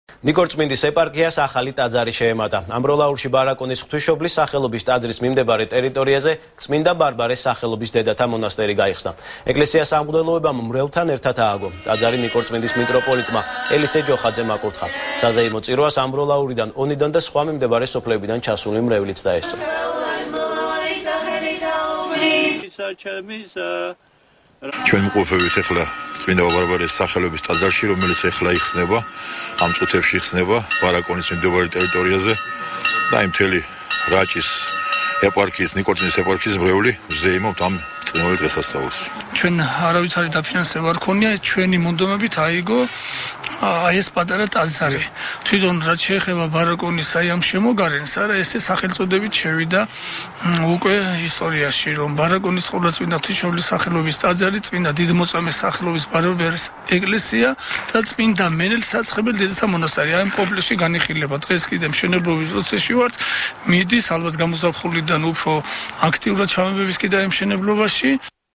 ახალი 6 | ეკლესია ბარაკონში | 17.12.12
ნიკორწმინდის ეპარქიას ახალი ტაძარი შეემატა. ამბროლაურში, ბარაკონის ღვთისმშობლის სახელობის ტაძრის მიმდებარე ტერიტორიაზე წმინდა ბარბარეს სახელობის დედათა მონასტერი გაიხსნა. ეკლესია სამღვდელოებამ მრევლთან ერთად ააგო.ტაძარი ნიკორწმინდის მიტროპოლიტმა ელისე ჯოხაძემ აკურთხა. საზეიმო წირვას ამბროლაურიდან,ონიდან და სხვა სოფლებიდან ჩასული მრევლი დაესწრო.